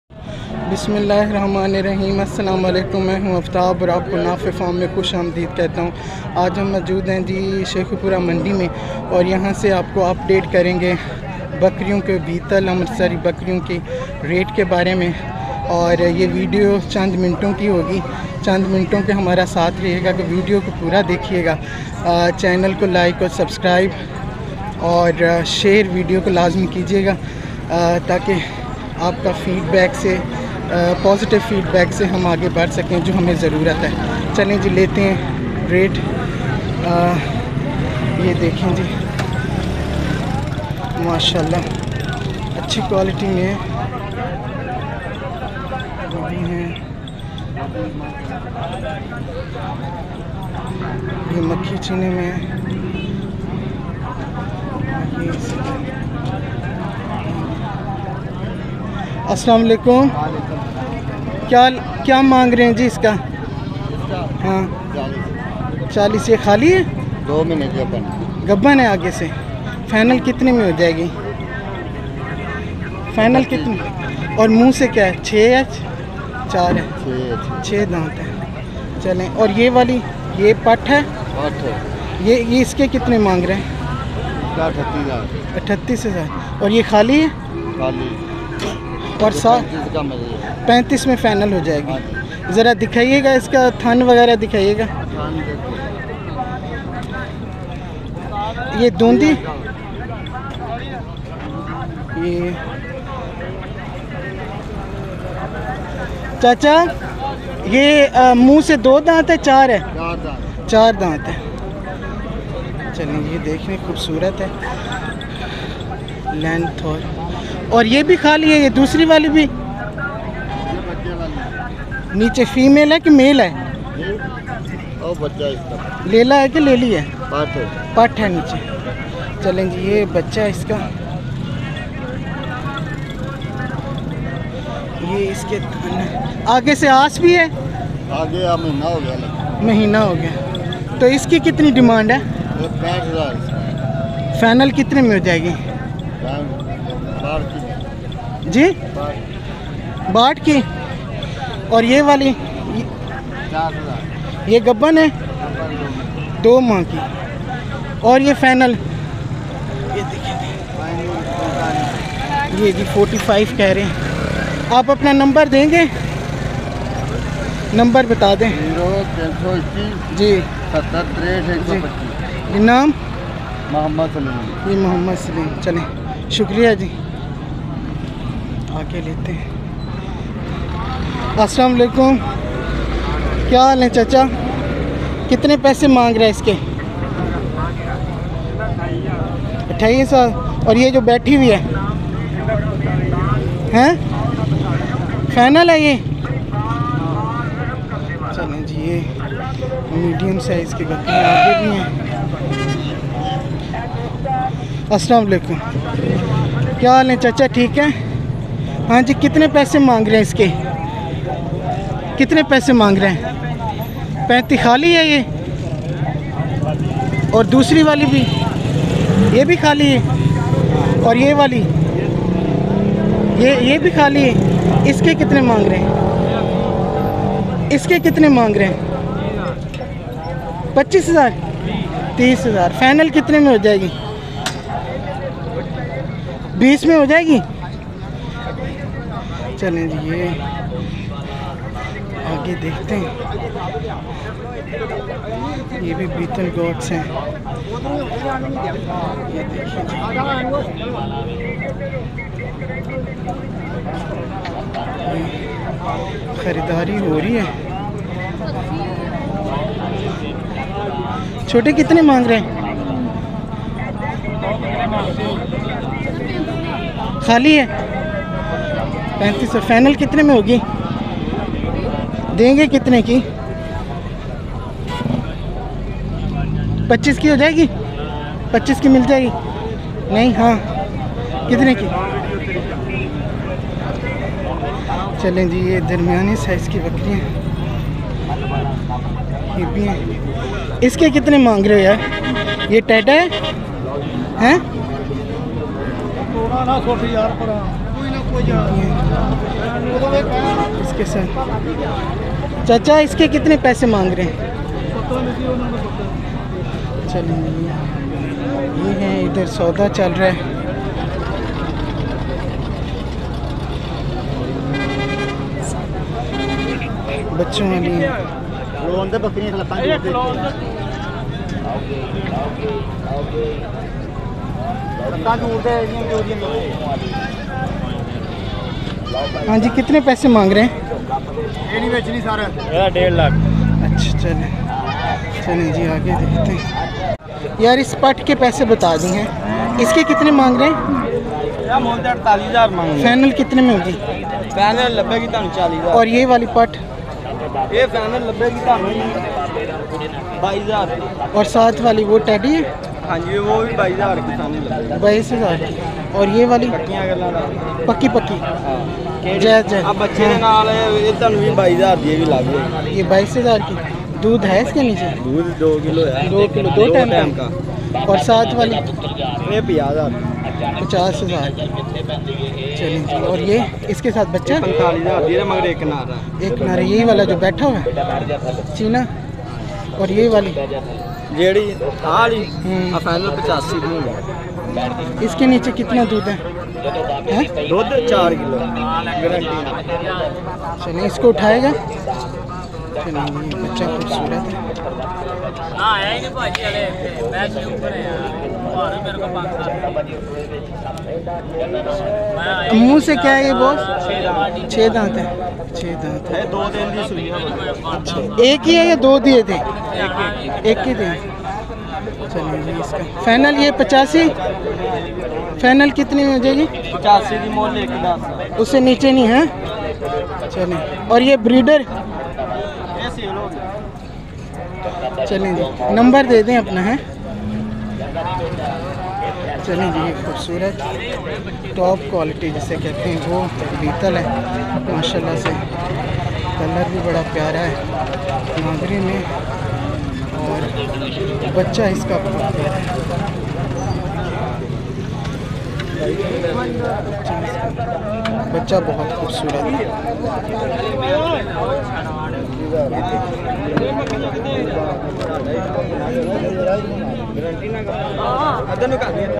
Top class Beetal goats in Sheikhupura Cattle Mandi,
Definitely we need your positive support

The Beetal goat is a breed from the Punjab region of India and Pakistan is used for milk and meat production. It is similar to the Jamnapari goat and the Malabari goat

Beetal goats are famous in below areas of Panjab - Pakistan
Faisalabad
Sahiwal
Pakpattan
Sheikhupura

Beetal goat

It is also known as Lahori goat; it is considered to be a good milker with large body size. Ears are flat long curled and drooping. The skin of these goats is considered to be of high quality because of its large size and its yielding of fine leather such as velour, suede, and chamois for manufacturing clothes, shoes, and gloves. Beetal goats have been widely used for improvement of local goats throughout the subcontinent. These goats are also adapted to stall feeding, thus are preferred for intensive goat farming.

Model Cattle Market Sheikhupura

Honorable Chief Minister of Punjab inaugurated Sheikhupura Model Cattle Market on April 2nd, 2015. Establishment of a Model Cattle Market in Sheikhupura was the first step towards achieving the goals of modern trading facility for sellers and buyers.

All basic facilities are provided by LDCMMC in the market.

Free services offered by LDCMMC in the market to general public, traders and farmers are water, electricity, veterinary services, shade (sheds), loading and unloading services, parking, security, cleanliness and toilets

Other available services offered through outsourcing in the market are fodder shops (tokka and without tokka), turri/paralli points, canteen and hotels, khokha and tea stalls, ornamental shops, lodging and boarding